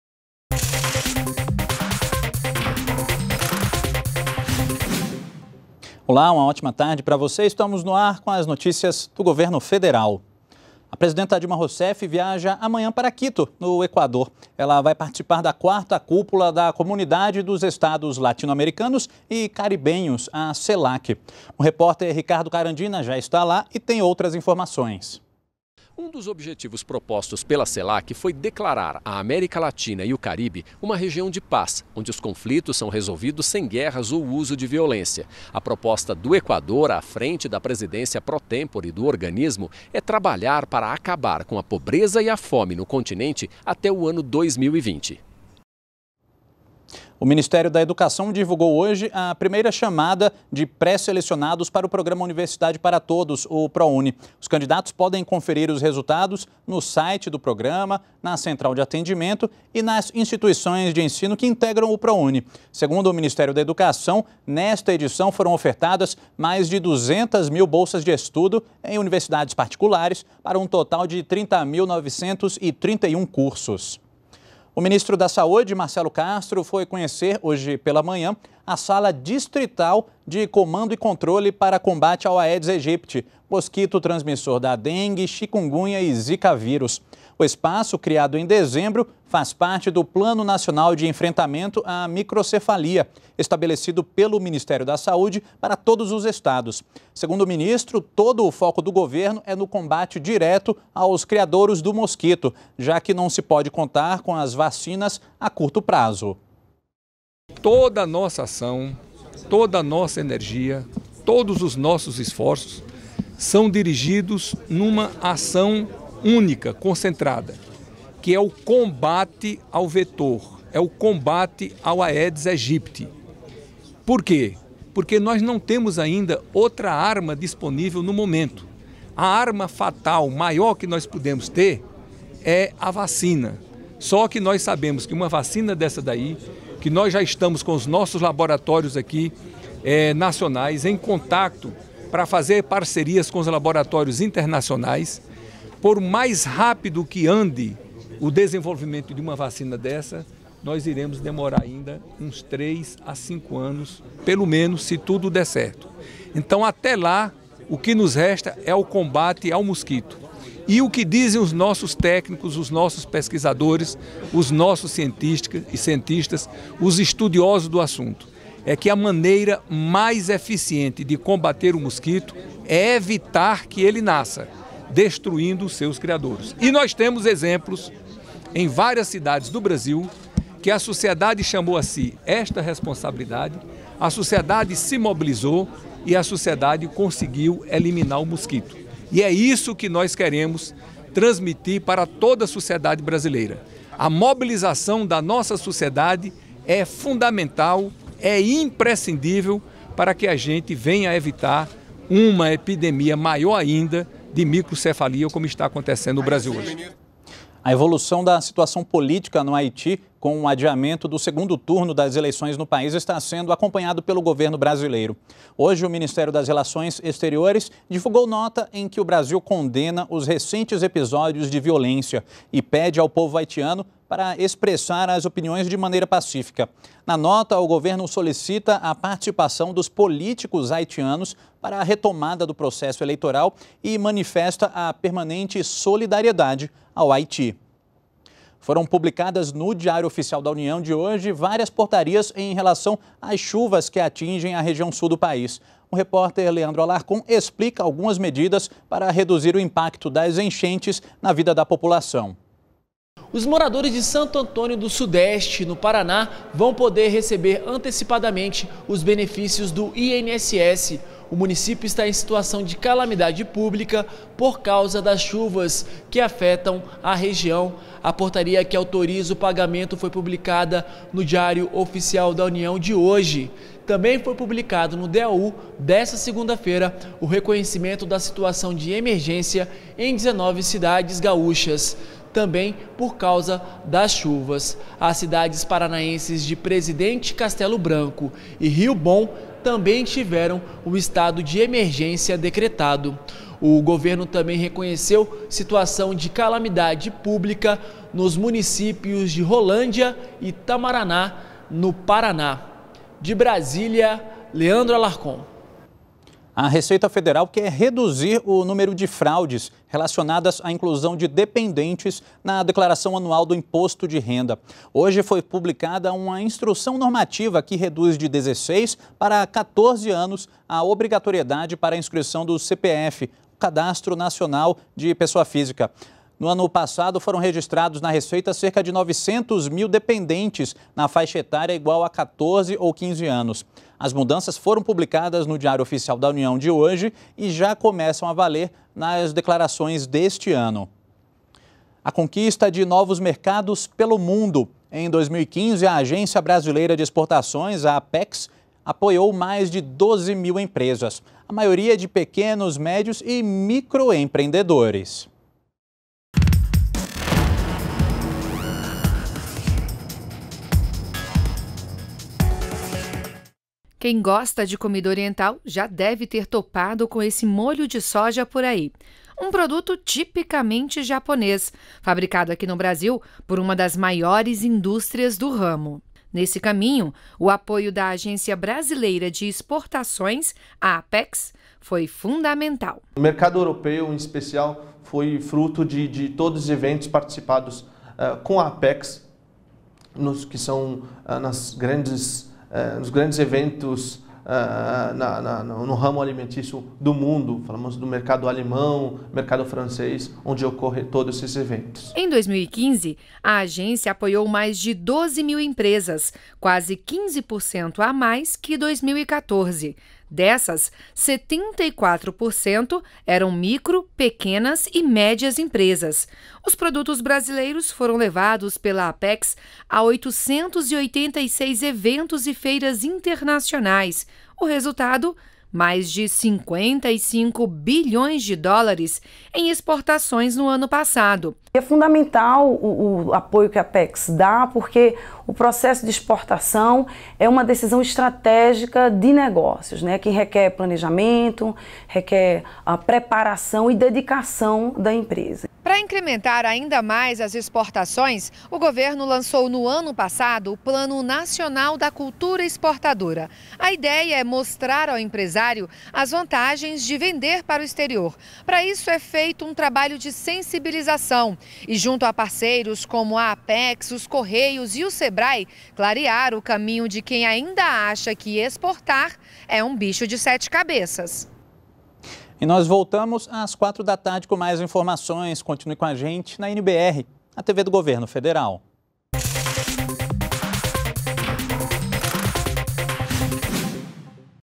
Últimas - Dilma Rousseff viaja amanhã para o Equador, onde participa da 4ª Cúpula da Celac
ÚLTIMAS - 25.01.16: Dilma Rousseff viaja nesta terça-feira (26), para Quito, capital do Equador, onde participará da 4ª Cúpula da Comunidade de Estados Latino-Americanos e Caribenhos (Celac). E mais: o Ministério da Educação divulgou, nesta segunda-feira (25), a primeira chamada de pré-selecionados para o Programa Universidade para Todos (Prouni). Veja também: ministro da Saúde, Marcelo Castro, visitou a sala distrital de comando e controle para combate ao Aedes Aegypti. Em entrevista, Castro falou que os esforços do governo são direcionados à erradicação dos focos de criação do mosquito, pois não é possível contar com uma vacina a curto prazo.